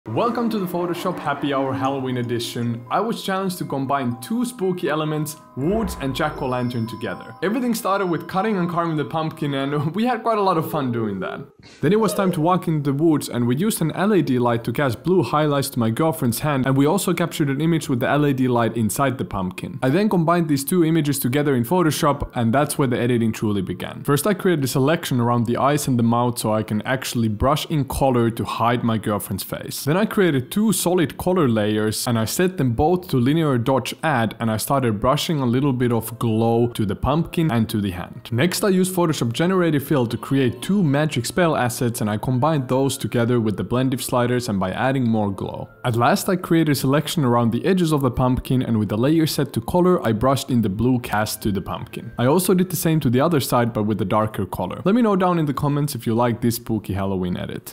Photoshop Happy Hour: Create a Glowing Jack-o’-lantern 🎃
This Halloween we challenged Photoshop artist Julius Kähkönen to create some spine-chilling artwork featuring the elements “Jack-o’-lantern” and “the woods.” Watch this tutorial to see how Julius transformed his girlfriend into a glowing monster in Photoshop. How’d he do?

Share your Halloween artwork on social with PsHalloween and MadewithPhotoshop.